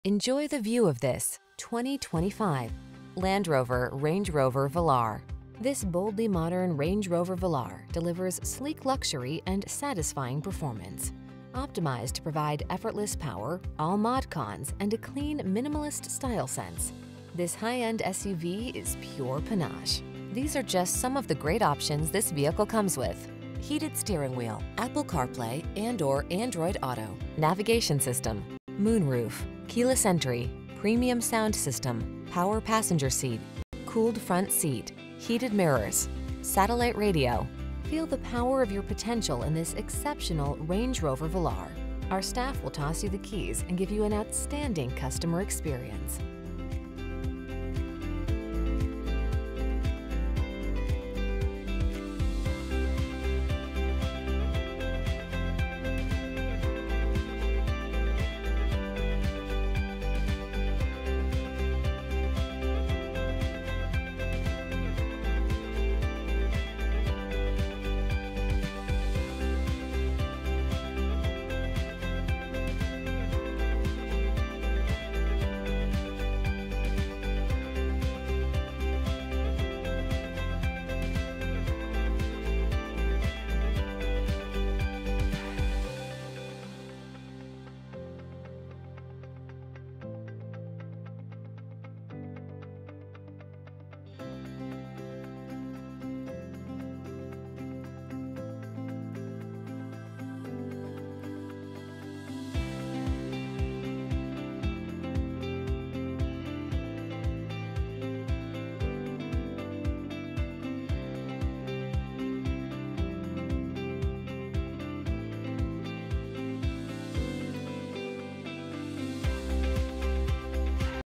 2025 Land Rover Range_Rover_Velar Dynamic SE Las Vegas, Henderson, North Las Vegas, Boulder Cit...
New 2025 Land Rover Range_Rover_Velar Dynamic SE available at Jaguar Land Rover Henderson located in 360 N Gibson Rd, Henderson, NV, 89014.
Stock#: R25030 - VIN#: SALYL2EX5SA803288

Servicing the Las Vegas, Henderson, North Las Vegas, Boulder City, Mesquite area.

Enjoy the view of this   2025  LAND ROVER Range Rover Velar. This boldly modern Range Rover Velar delivers sleek luxury and satisfying performance. Optimized to provide effortless power, all mod cons, and a clean, minimalist style sense, this high-end SUV is pure panache. These are just some of the great options this vehicle comes with: Heated Steering Wheel
, Apple Carplay®/Android Auto®, Navigation System, Moonroof, Keyless Entry, Premium Sound System, Power Passenger Seat, Cooled Front Seat(s), Heated Mirrors, Satellite Radio. Feel the power of your potential in this exceptional Range Rover Velar. Our staff will toss you the keys and give you an outstanding customer experience.